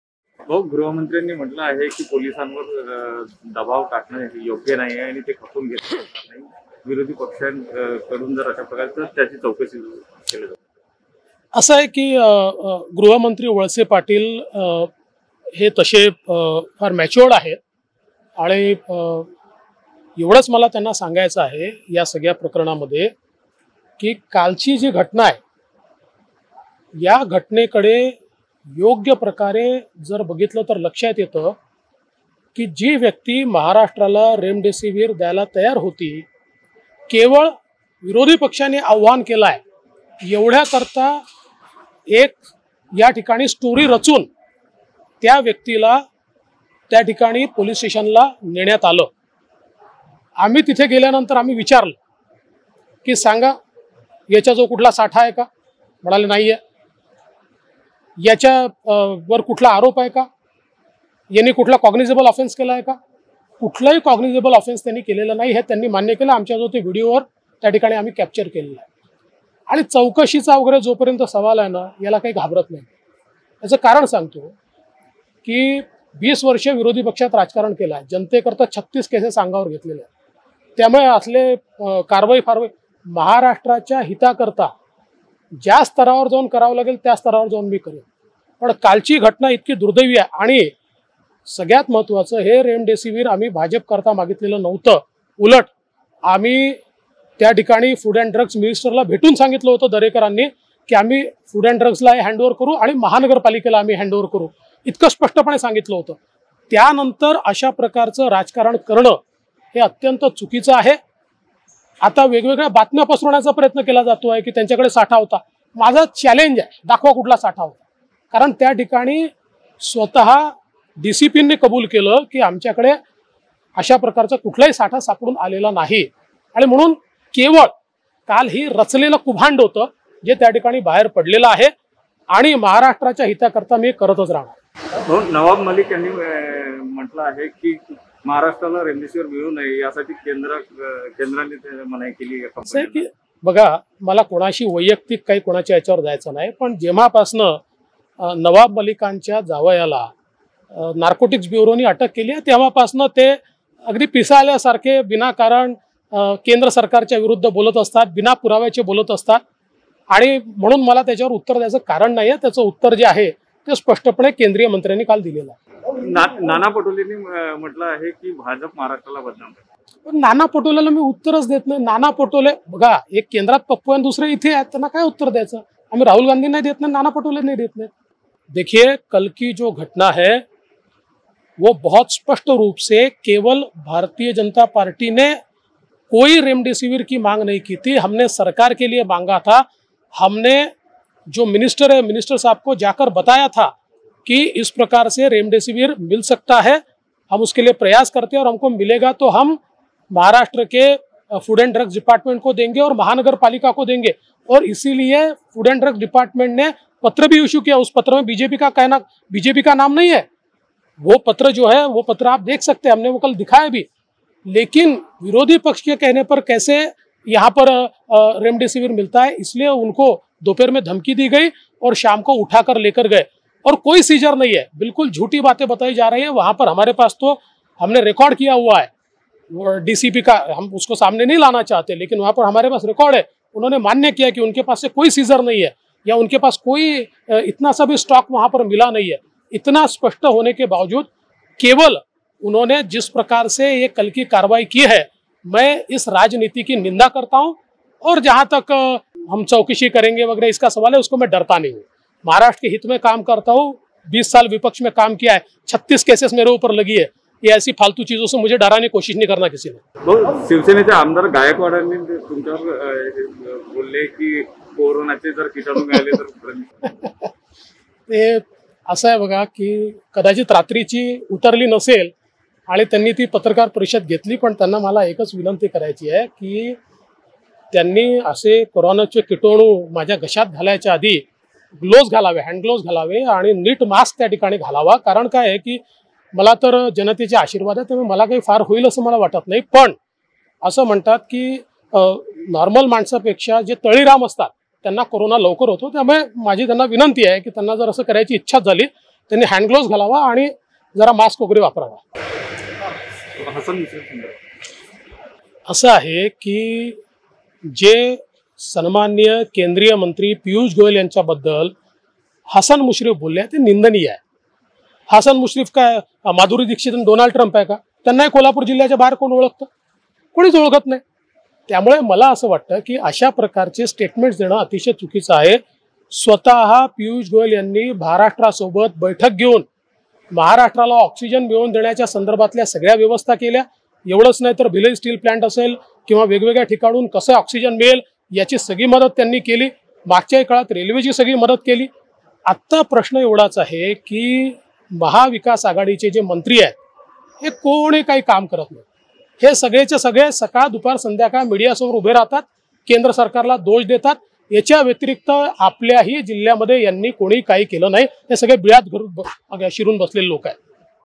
महाराष्ट्र हितासाठी सर्व प्रयत्न करणार ! विरोधी पक्षनेते देवेंद्र फडणवीस यांचा माध्यमांशी संवाद
अफवा पसरवू नका!
रेमडेसिवीर भाजपसाठी नाही, महाराष्ट्रासाठी मागत होतो, म्हणूनच मंत्र्यासोबत बैठक झाली.
ब्रूक फार्मा कंपनीकडे साठा असेल तर पुरावे द्या!
बाकी धमक्यांना मी घाबरत नाही, जनतेसाठी कितीही केसेस लागल्या तरी त्याची आपण पर्वा करत नाही! : श्री देवेंद्र फडणवीस
18 April 2021

0:00 - चौकशीला आम्ही घाबरत नाही!
आम्ही कोणाला मदत केली नाही. निव्व्ळ विरोधी पक्षाने केले म्हणून विरोध. संबधित व्यक्तीवर कोणताच गुन्हा नाही. 20 वर्षे महाराष्ट्रात विरोधी पक्षात राजकारण, जनतेच्या हिताकरिता 36 केसेस लढल्या. राज्याच्या हिताकरिता प्रयत्न करणार, कोणत्याही चौकशीला घाबरत नाही.

1:31 - रेमडेसिवीर भाजप करता मागितलेच नव्हते!
रेमडेसिवीर हे जनतेसाठीच, ते पक्षासाठी नव्हतेच, त्यावरुन राजकारण करु नये.

1:52 - हे रचलेले कुभांड होते!
रेमडेसिवीरचा साठा असल्याच्या बातम्या पसरवल्या, त्याचा पुरावा काय आहे? उपायुक्तांकडूनही कोणताही साठा सापडला नसल्याचे स्पष्ट. हे केवळ एक नाटक, महाराष्ट्राच्या हितासाठी प्रयत्न करतच राहणार.

2:15- विनाकारण केंद्र सरकारच्या विरुध्द बोलायचे
नवाब मलिक विनाकारण आणि विना पुरावा केंद्र सरकारच्या विरोधात बोलतात, त्याचे स्पष्टीकरण काल केंद्रीय मंत्र्यांनी दिले.

2:46 - एक केंद्रात पप्पू आहे आणि दुसरा इथे!
नाना पटोले काहीही बोलतात, त्यांनी उत्तर देणार नाही. आम्ही राहुल गांधींनाही देत नाही आणि नाना पटोलेंनाही देणार नाही.

4:20 - मला तर जनतेचे आशीर्वाद!
शिवसेनेच्या आमदारांची टीका हास्यास्पद. सामान्य माणसापेक्षा तळीरामांना कोरोनाची शक्यता अधिक, त्यांनी हॅण्ड ग्लोव्हज आणि मास्क घालावा.

5:03 - महाविकास आघाडीचे मंत्री हे बिळात शिरुन बसलेले!
हसन मुश्रीफांनी मंत्री पियुष गोयलांवर केलेली टीका चुकीची. गोयल यांनी महाराष्ट्राला कोरोनाच्या काळात विविध स्तरावर मदत, ऑक्सिजनसाठी सहकार्य, महाविकास आघाडीचे मंत्री निष्क्रीय, बिळात शिरुन बसल्यासारखेच.